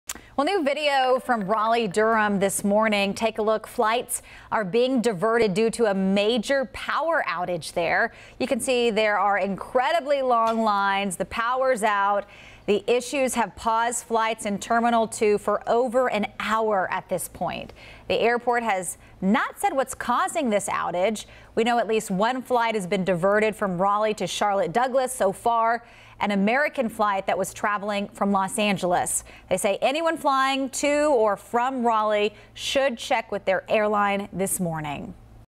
Power outage shuts down terminal at Raleigh-Durham airport
Raleigh-Durham International Airport is dealing with a major power outage Friday morning.

The outage is so severe that RDU said it was unable to serve passengers in Terminal 2, the airport's busiest terminal. Terminal 1, which services Southwest Airlines, was unaffected.

A spokesperson at the airport said the cause was due to a water leaking onto an electrical box. The power outage was impacting all systems, including ticketing, security and gate bridges.

A contractor was on the way to repair the electrical outage, officials said.